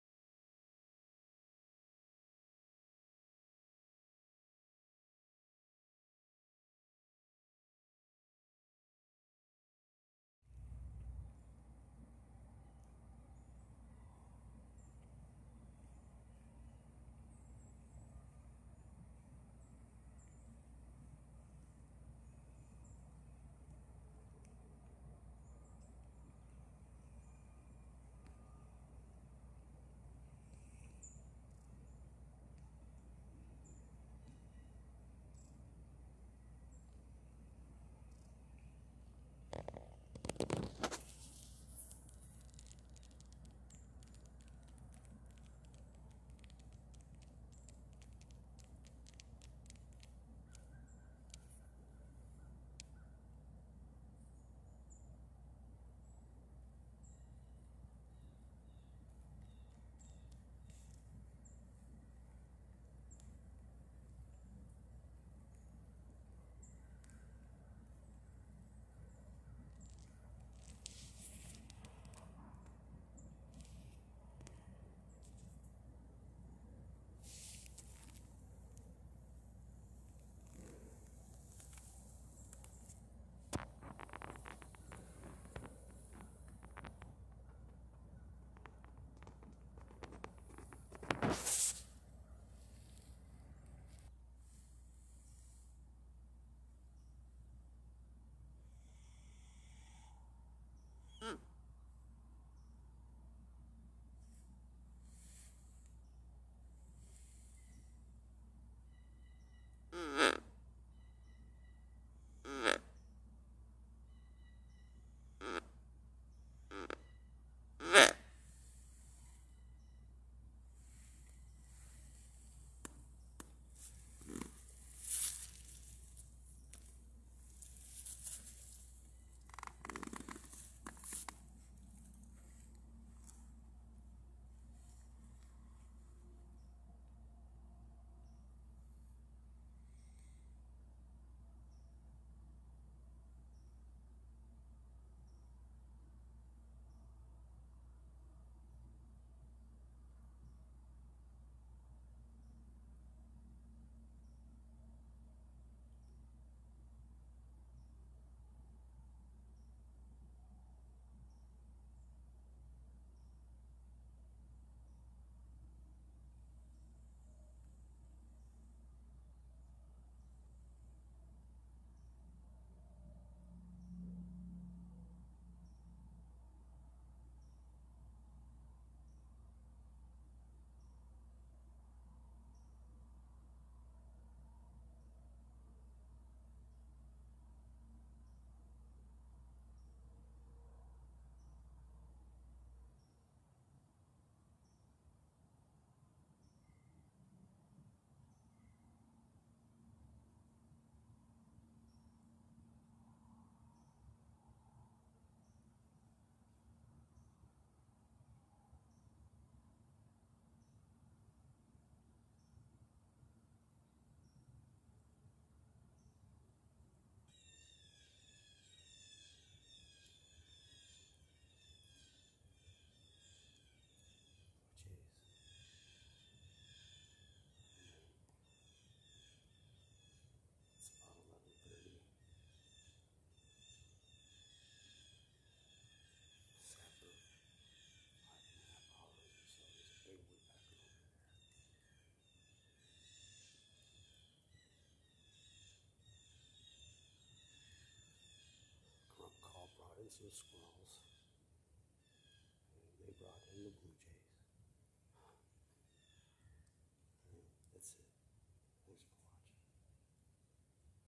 Bow Hunt 11/9
This years getting frustrating. Not even seeing deer. It is what it is. As I said in the about section, it's sharing the journey, the failures and success.